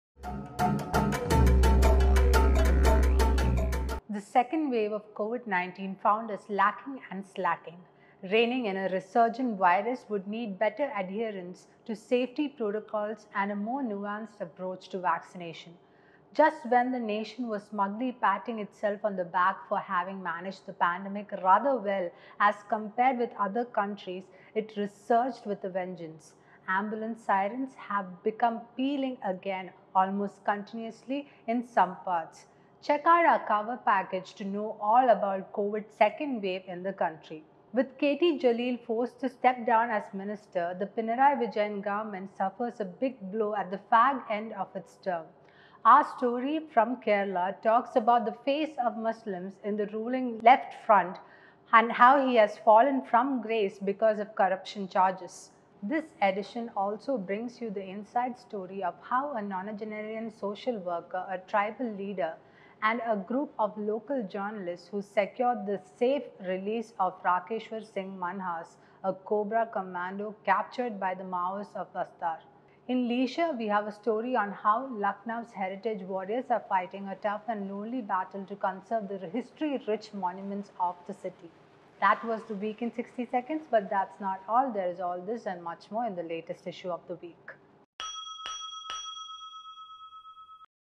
Second wave of Covid-19 | K. T. Jaleel’s fall | Heritage warriors | THE WEEK in 60s
Just when the nation was smugly patting itself on the back for having managed the pandemic rather well as compared with other countries, it resurged with a vengeance.